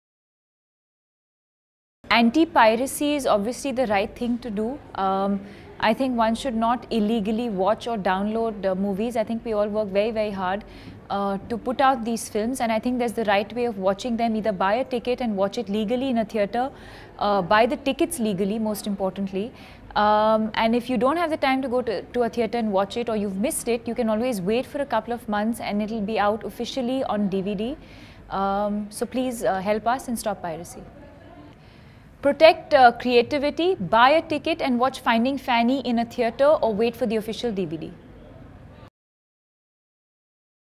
Deepika Padukone on Anti-Piracy.
Deepika Padukone asks you to only watch Finding Fanny in theatres and authorised DVDs, and boycott pirated prints of the film.
Finding Fanny releases on 12th September worldwide.